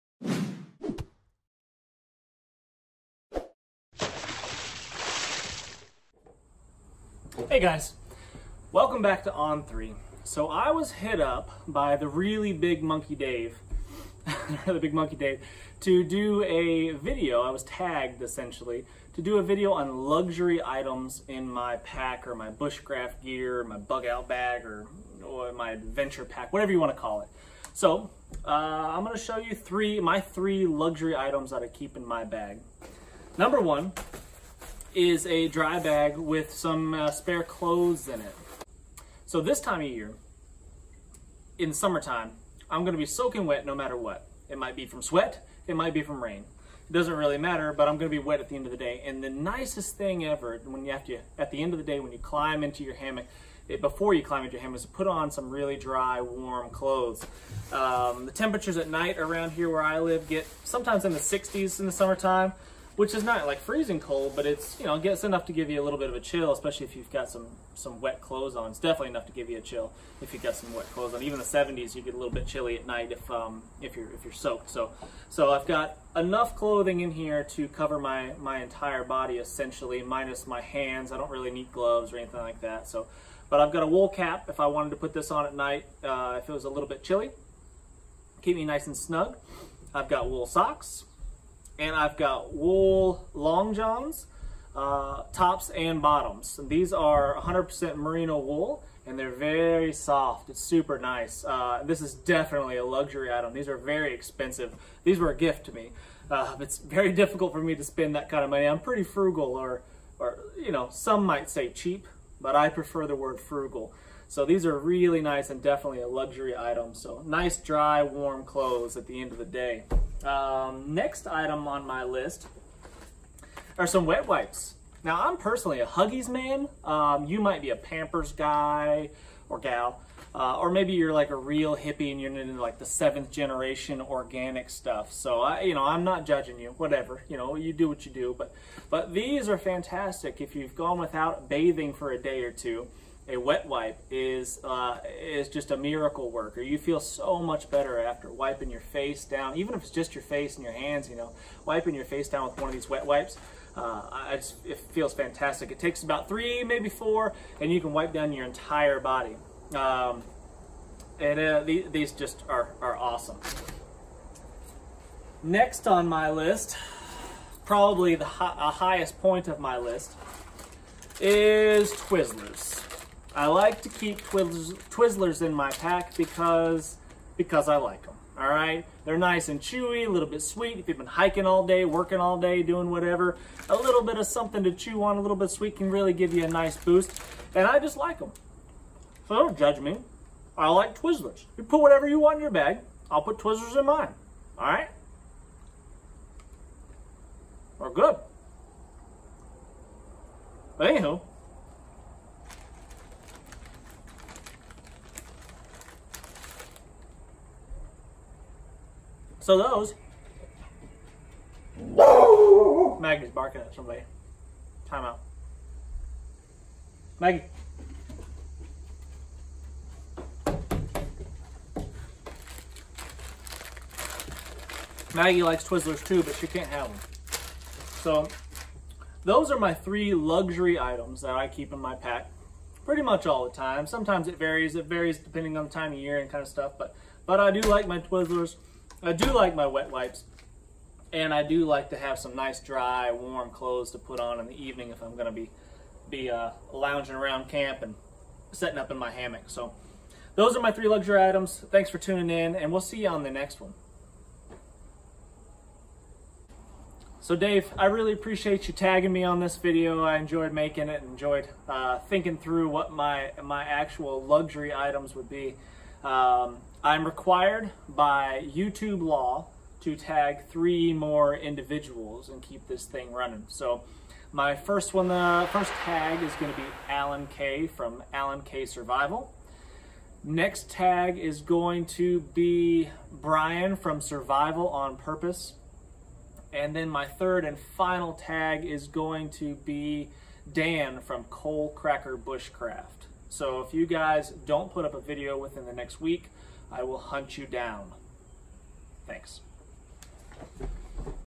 Luxury Items In My Bushcraft Bugout Bag Tag
to show my 3 luxurious items I keep in my pack.
I have tagged three others.